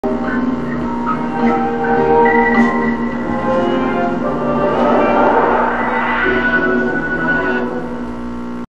Vinheta: RBS TV 40 Anos (2002)
Créditos: Paolo Pereira Zan (detentor da conta /ppzan)

Vinheta comemorativa dos 40 anos da RBS TV, exibida em 2002.